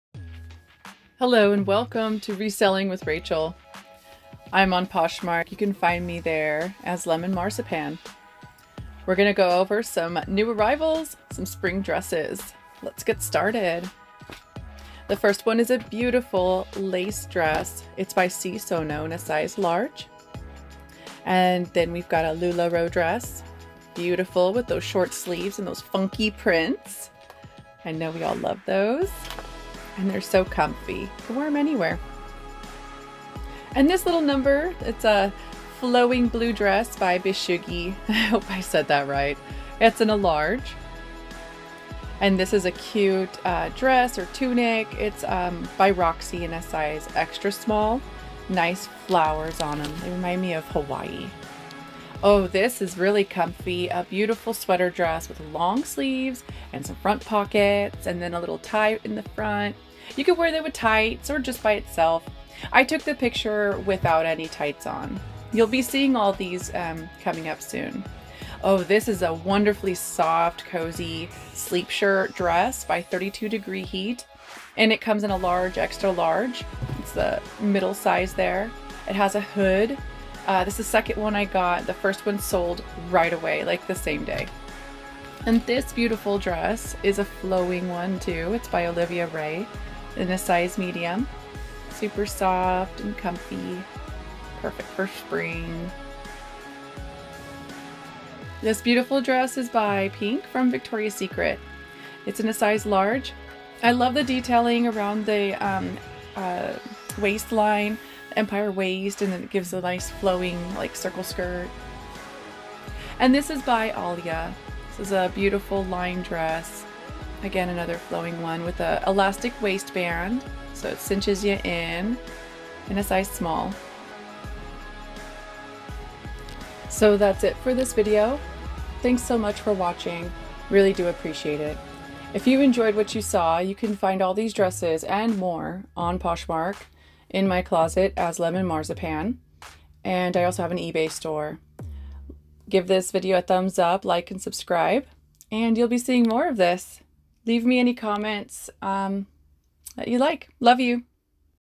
New Arrivals To Lemon Marzipan Closet on Poshmark App SPRING DRESSES May 21 2021
new dress arrivals for Spring May 2021 for sale on Poshmark in the
 lemon marzipan clothing shop closet